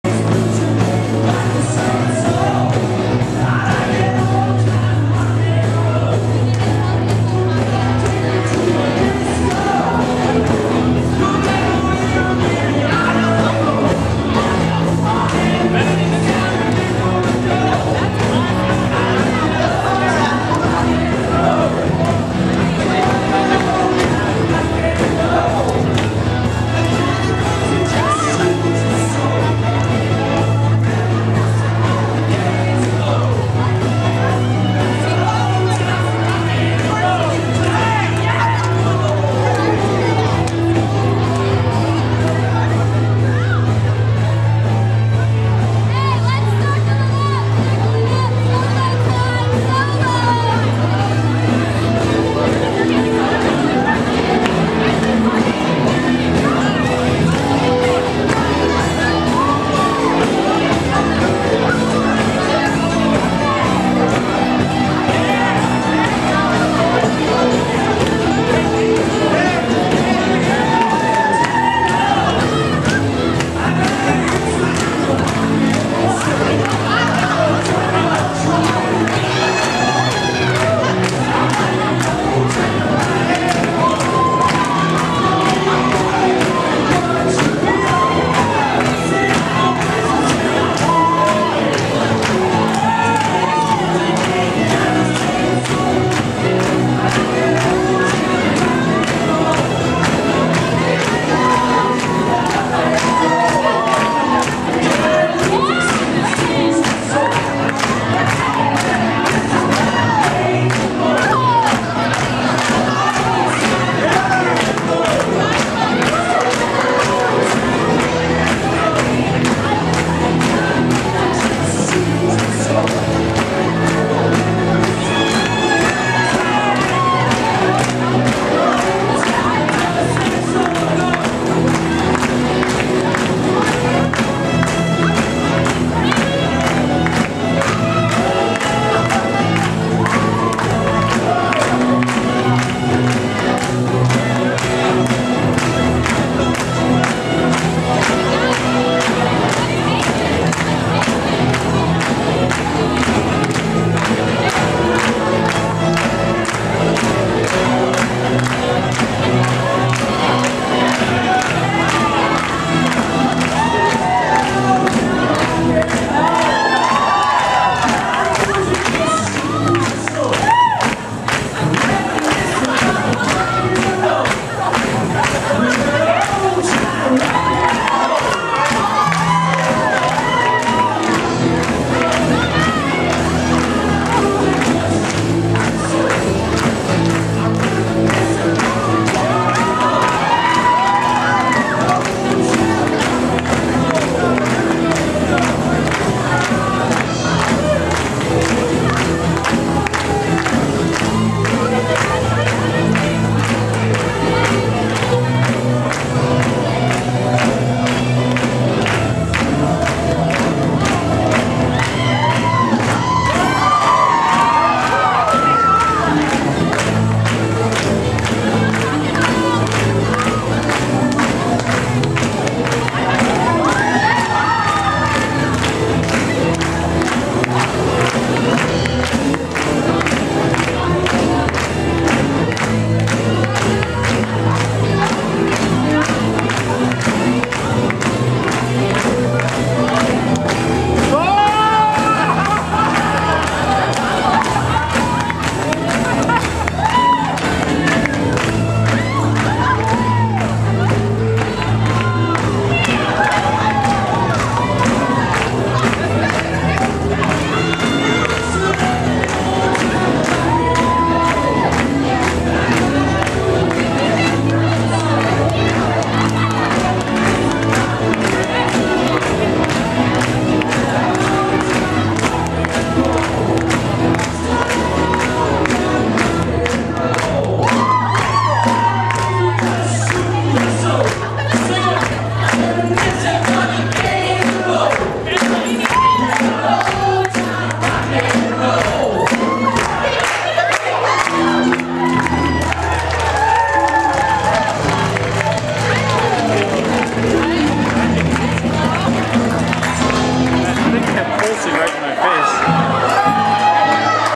Solo Dancing - Andrew and Katelyn Meyer's Wedding!!
The fantastic solo dancing efforts at Andrew and Katelyn's wedding on 7/30/11.  Great times everyone!